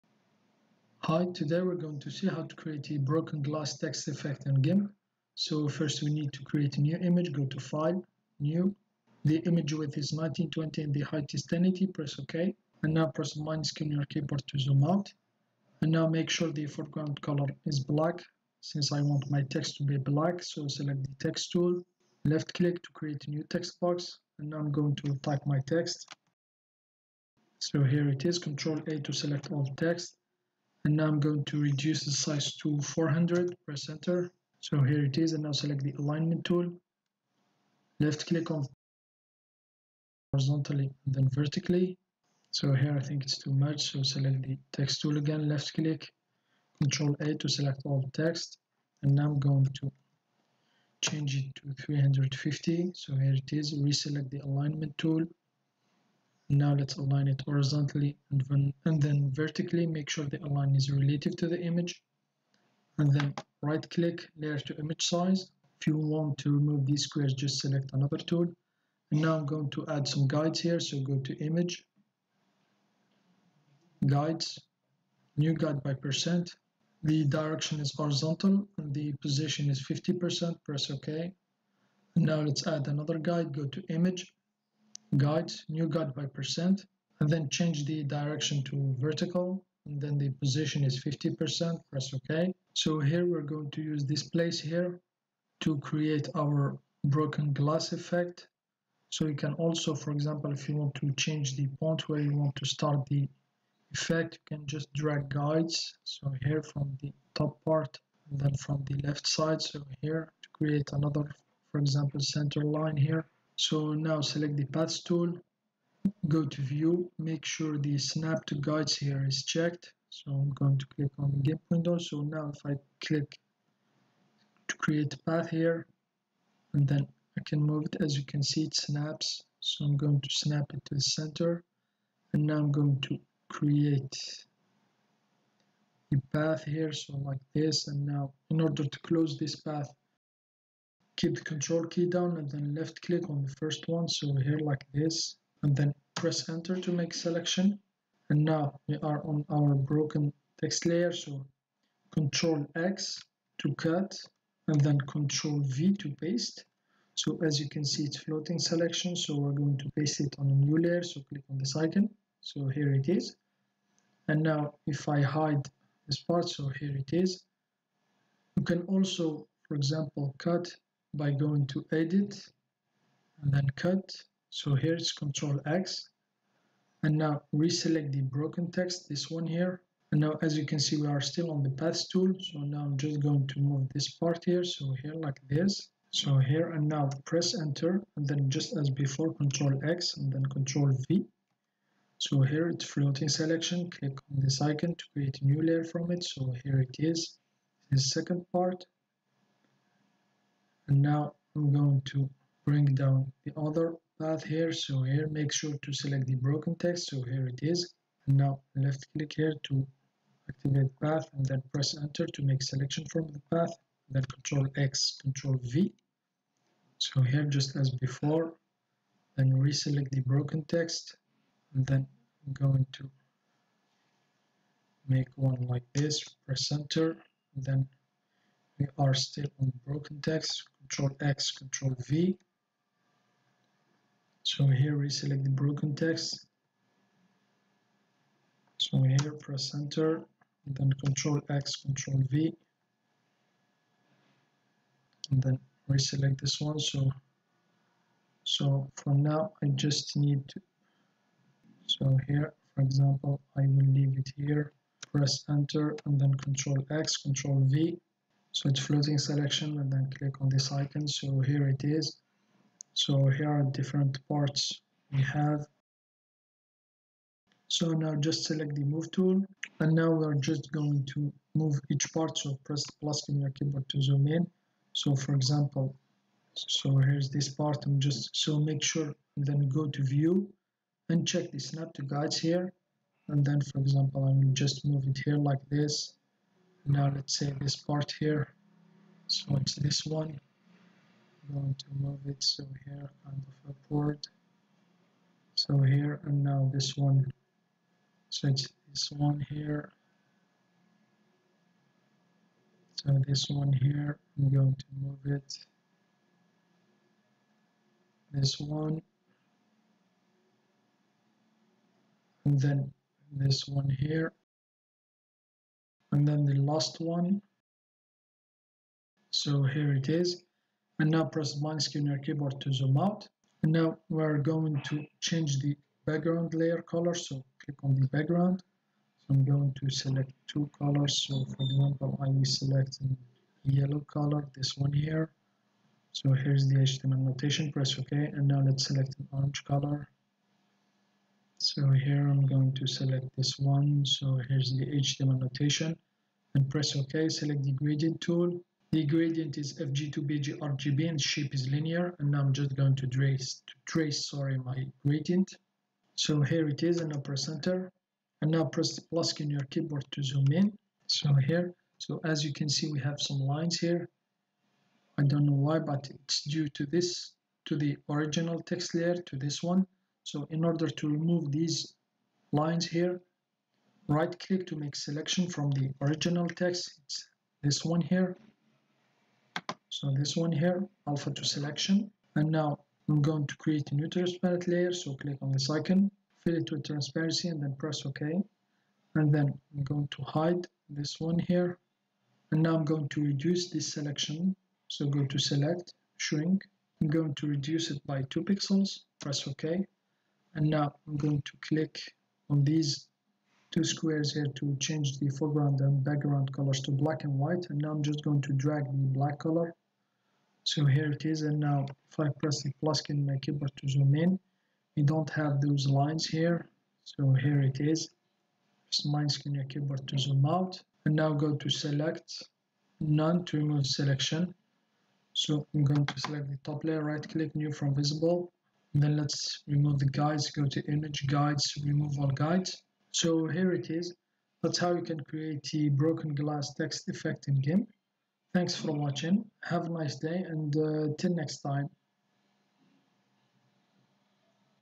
Broken Glass Text Effect in GIMP
Hi, today, we are going to see how to create a Broken Glass Text Effect in GIMP.